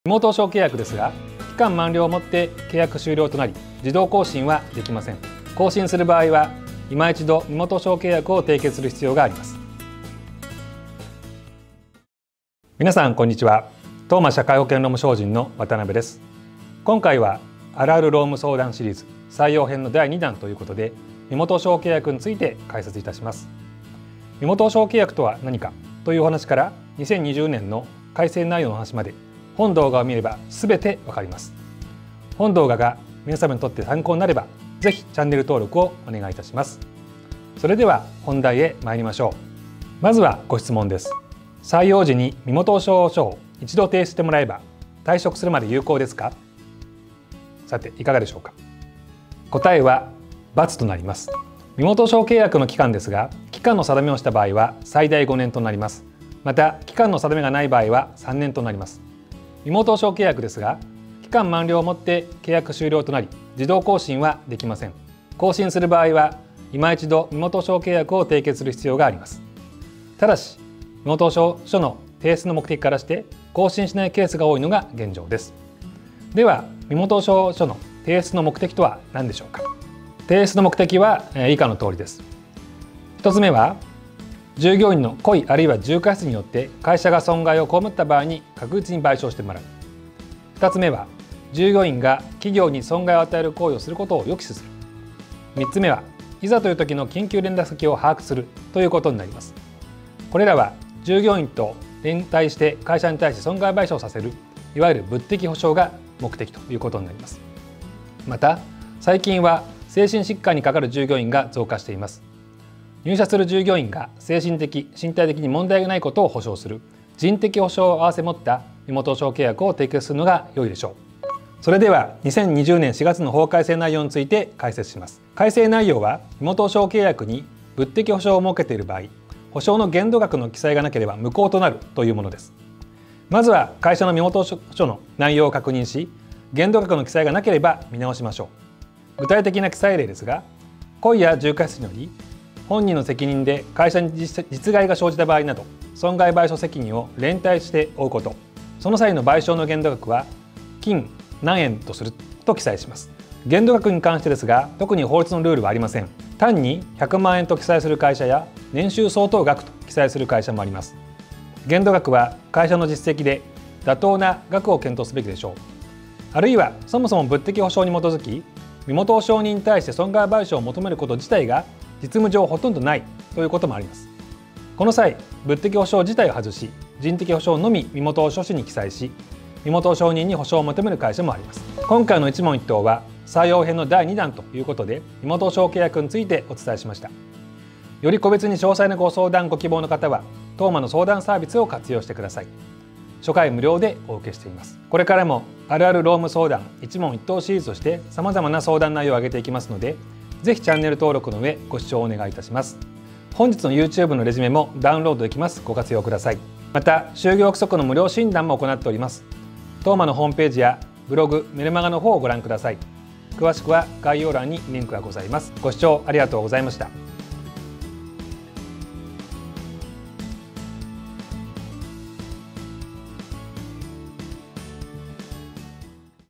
【更新し忘れ注意！】これ一本で必要な対応がわかります！意外と見落としがちな身元保証契約の話【社労士法人監修】
「これ一本で必要な対応がわかります！意外と見落としがちな身元保証契約の話」
今回は、あるある労務相談シリーズ、採用編の第二弾ということで、身元保証契約について解説いたします。
そもそものお話から、2020年に法改正があった内容までこの動画を見ればすべてわかります。

【メルマガ　経営者のための人事・労務の必要最低知識】

【チャプター】
00:00　プレビュー
00:12　はじめに
00:41　身元保証契約の期間
01:20　身元保証書提出の目的
02:01　法改正内容
03:02　TOMAにお任せください！

【レジュメダウンロード】
準備中